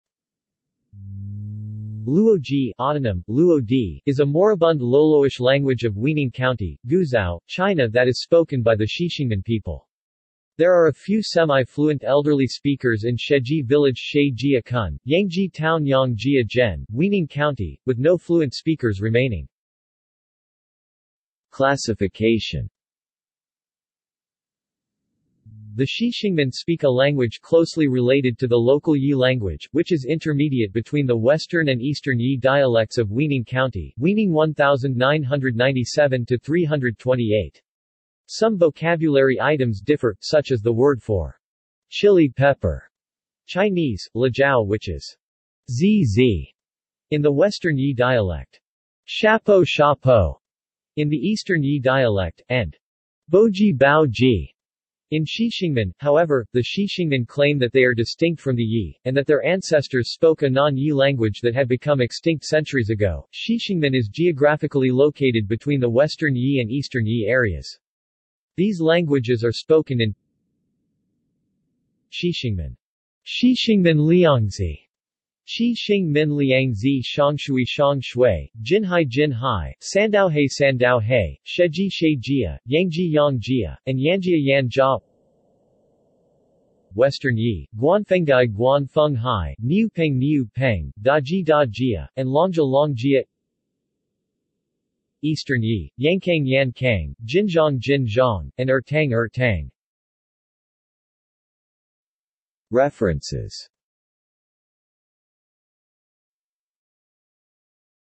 Luoji language | Wikipedia audio article
This is an audio version of the Wikipedia Article:
Luoji language

SUMMARY
Luoji (autonym: luo˧˩dʑi˧) is a moribund Loloish language of Weining County, Guizhou, China that is spoken by the Qixingmin people. There are a few semi-fluent elderly speakers in Shejie Village 蛇街村, Yangjie Town 羊街镇, Weining County, with no fluent speakers remaining.